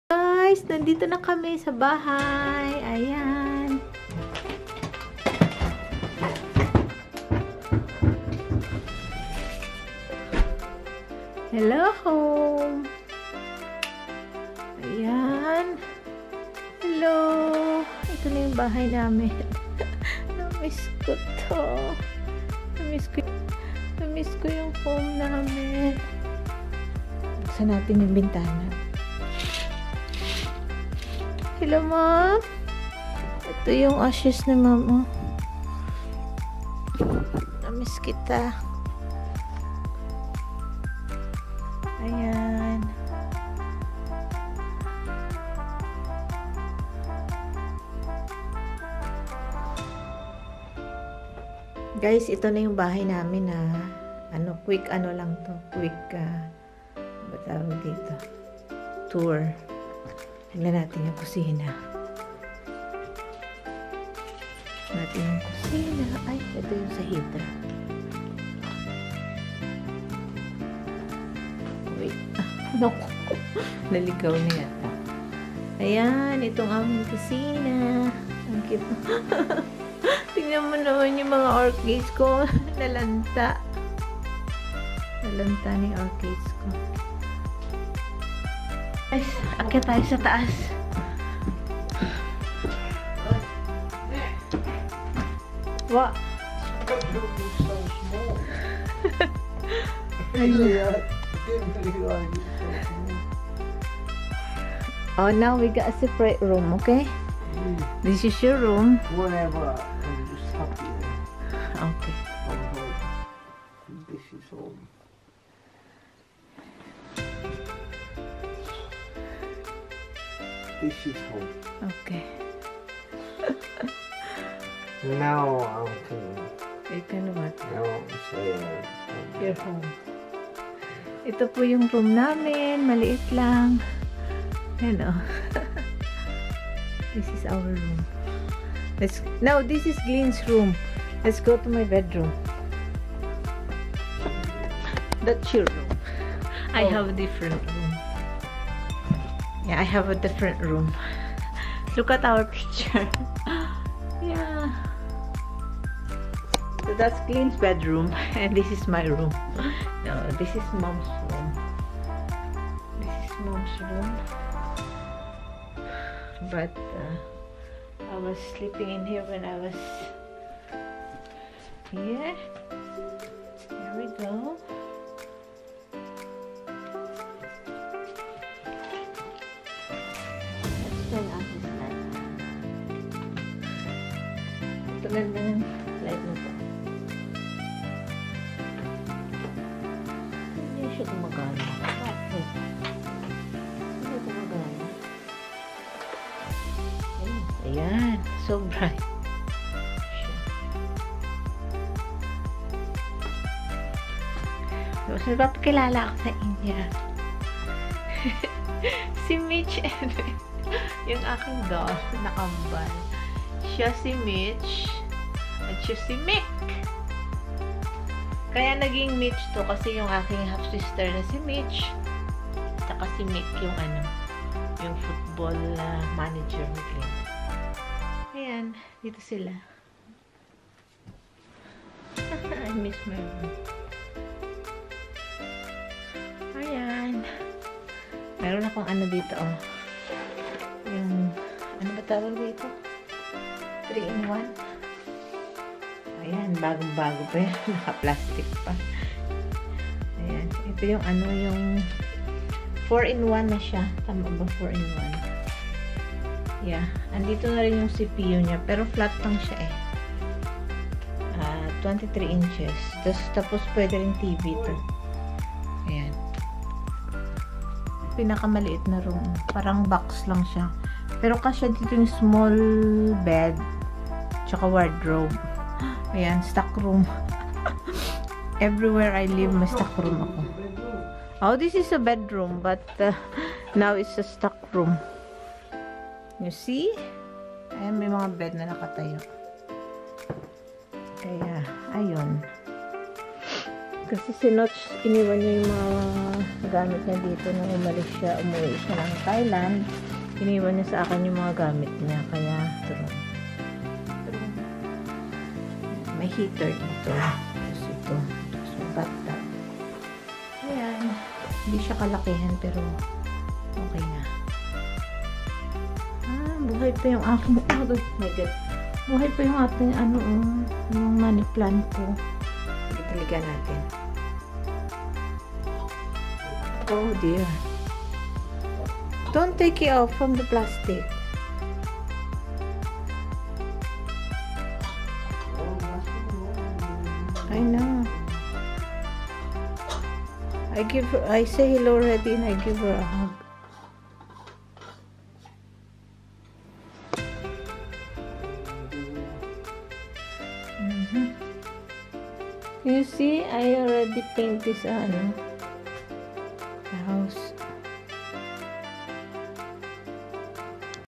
British-Filipina Expat (House Tour in the UK)
Hi mga friends the quick house tour na eto ay straight from the airport pagdating nmin, I'm just so excited that why I'm sharing it to you! Maliit na bahay eto pero tamang tama na sa aming dalawa ng asawa ako.